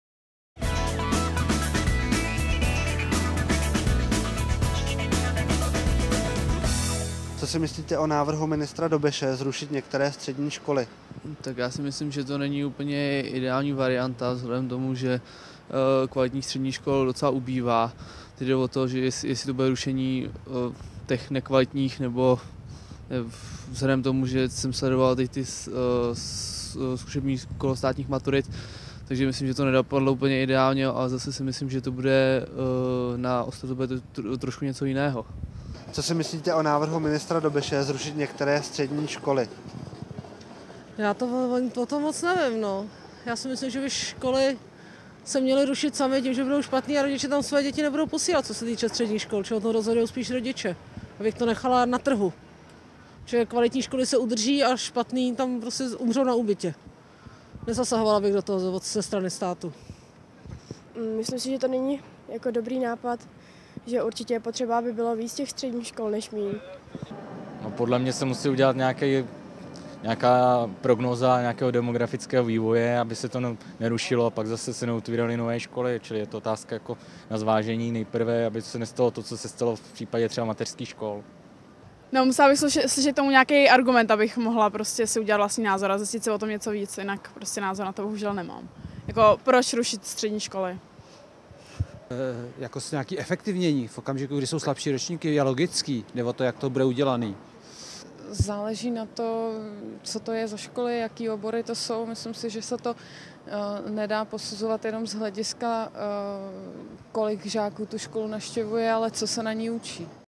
Anketa: Co si myslíte o návrhu ministra Dobeše zrušit některé střední školy?
Ministr školství Josef Dobeš chce zrušit některé střední školy. Hlavním důvodem pro tento krok je argument, že je málo středoškoláků a učebny zůstávají poloprázdné a jejich provoz se nevyplatí. Při rozhodování jaké školy zrušit, by měly pomoci výsledky státních maturit. Zeptali jsme se občanů, co si myslí o tomto návrhu ministra školství.
Většina mladých lidí, které jsme na ulici oslovili, nesouhlasí se zrušením některých středních škol. Jeden respondent argumentoval tím, že kvalitních středních škol stále ubývá a není to rozhodně dobrý nápad. Někteří se obávají, že by to mohlo dopadnout jako při rušení mateřských škol a nyní nejsou pro předškoláky volná místa. Podle jednoho respondenta je jisté zefektivnění provozu v době slabších ročníků logické, ale je důležité sledovat, jakým způsobem je to provedeno.